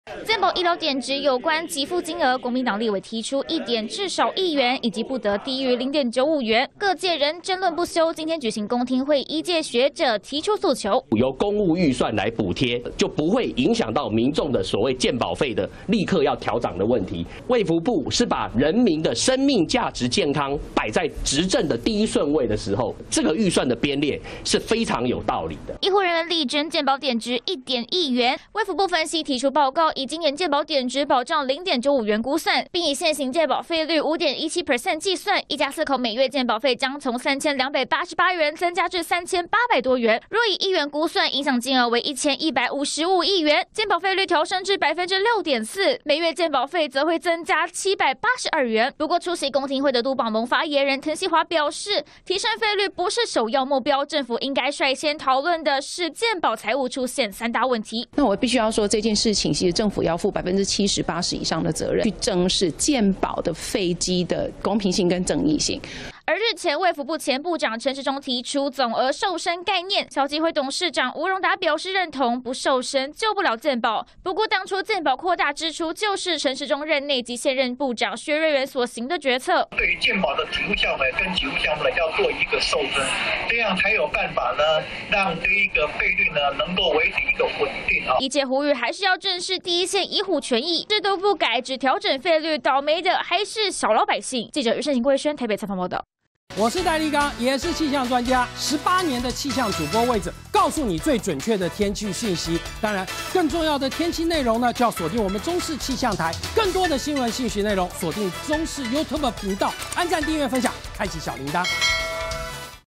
"健保1點1元"明年狂漲健保?! 專家批:政府應負70%責任│中視新聞 20240506
健保"保障點值"近來成為關注焦點，今天召開公聽會，醫師代表希望爭取一點換一元, 但學者憂心沒有足夠配套, 等同廢除總額制度，不只政府財政扛不起來，恐怕只能走向提高健保費一途!專家則認為應該從制度上改革，而不是一昧調漲費率。